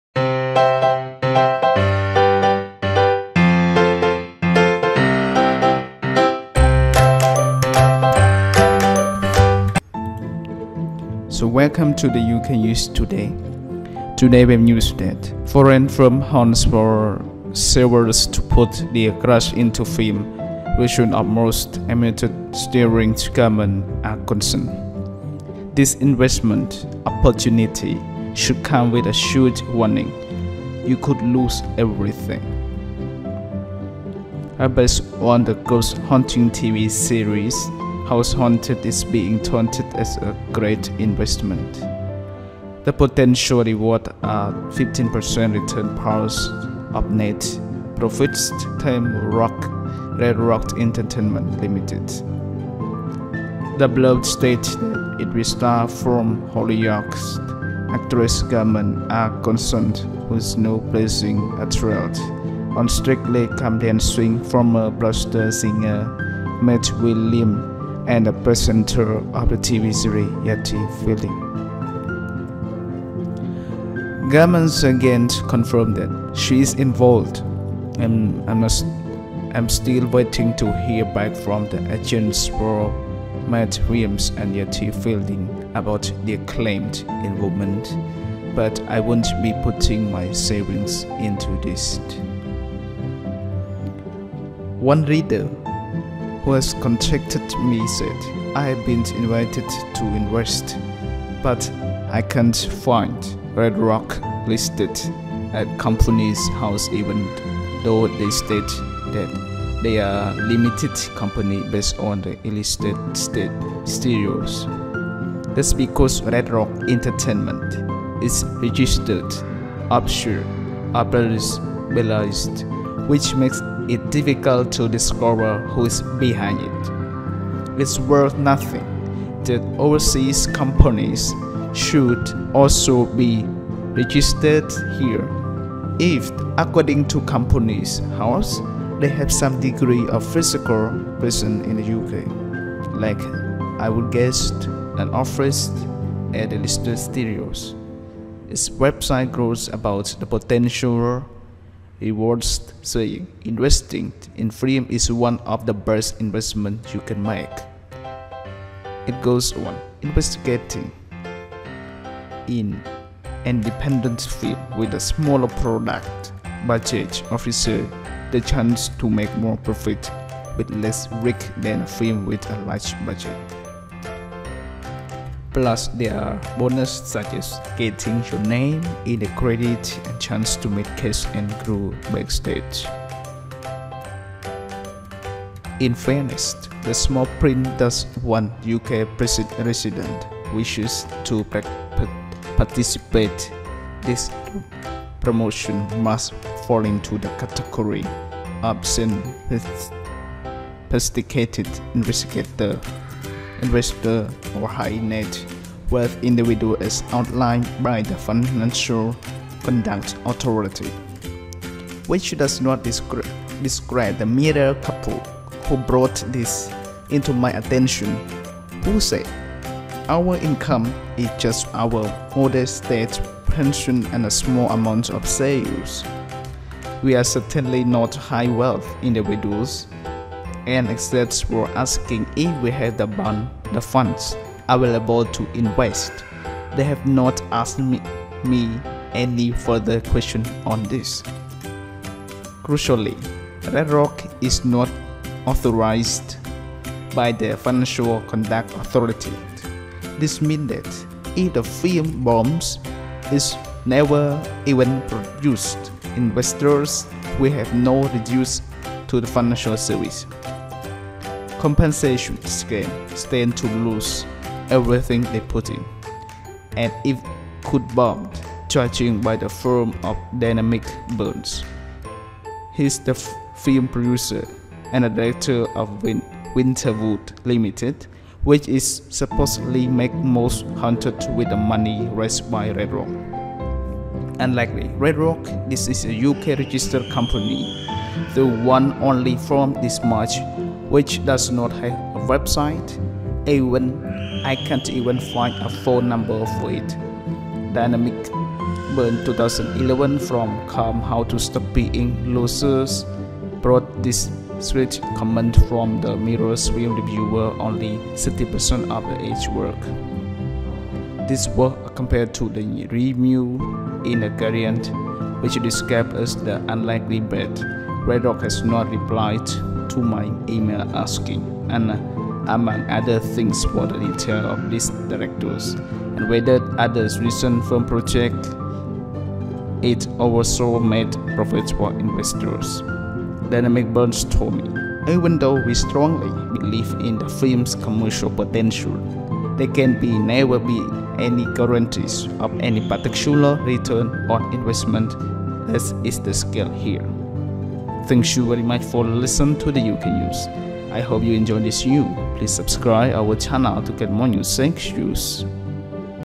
daily uk news:Foreign firm hunts for savers to put their cash into film
daily uk news:Foreign firm hunts for savers to put their cash into film version of Most Haunted starring Gemma Atkinson
A film based on the ghost-hunting tv series Most Haunted is being touted as a great investment.
The potential rewards are a 15% return plus 50% of net profits, tempts Red Rock Entertainment Limited.
The blurb states that it will star former Hollyoaks actress Gemma Atkinson, who's now blazing a trail on Strictly Come Dancing, former Buster singer Matt Willis and the presenter of the tv series Yvette Fielding.
Gemma's agent confirmed that she is involved and I'm still waiting to hear back from the agents for Matt Willis and Yvette Fielding about their claimed involvement, but I won't be putting my savings into this.
Yvette Fielding and Matt Willis (Image: Red Rock Entertainment)
One reader who has contacted me said: “I have been invited to invest but I can't find Red Rock listed at Companies House even though they state that they're a limited company based at Elstree Studios.”
That's because Red Rock Entertainment is registered offshore in Belize, which makes it difficult to discover who is behind it.
It's worth nothing that overseas companies should also be registered here if, according to Companies House, “they have some degree of physical presence in the UK” - like, I'd guess, an office at Elstree Studios.
Its website crows about the potential rewards, saying: “Investing in film is one of the best investments you can make."
It goes on: “Investing in an independent film with a smaller production budget offers the chance to make more profit with less risk than a film with a large budget.”
Plus, there are bonuses such as getting your name in the credits and the chance to meet the cast and crew backstage.
In fairness, the small print does warn: “UK residents wishing to participate in this promotion must fall into the category of sophisticated investor or high net worth individual as outlined by the Financial Conduct Authority”.
Which does not describe the Mirror couple who brought this to my attention, who said: “Our income is just our modest state pension and a small amount of savings.
"We are certainly not high wealth individuals and except for asking if we have the funds available to invest they have not asked me any further questions on this."
Crucially, Red Rock is not authorised by the Financial Conduct Authority. This means that if the film bombs, or is never even produced, investors will have no recourse to the Financial Services Compensation Scheme and stand to lose everything they put in.
And it could bomb, judging by the form of Dominic Burns.
He’s a film producer and a director at Winterwood Limited, which will supposedly make Most Haunted with the money raised by Red Rock. Unlike Red Rock, this is a UK registered company, though one only formed this March which doesn't have a website and I can't even find a phone number for it.
Dominic Burns' 2011 rom-com How To Stop Being A Loser brought this scathing comment from the Mirror’s film reviewer: “Only about 30% of the gags work.”
That was kind compared to the review in The Guardian, which described it as “unrelentingly bad”.
Red Rock has not replied to my emails asking, among other things, for the details of its directors and whether other recent film projects it oversaw made a profit for investors.
Dominic Burns told me: "Although we strongly believe in the film's commercial potential, there can never be any guarantees of any particular return on investment, as is the case here."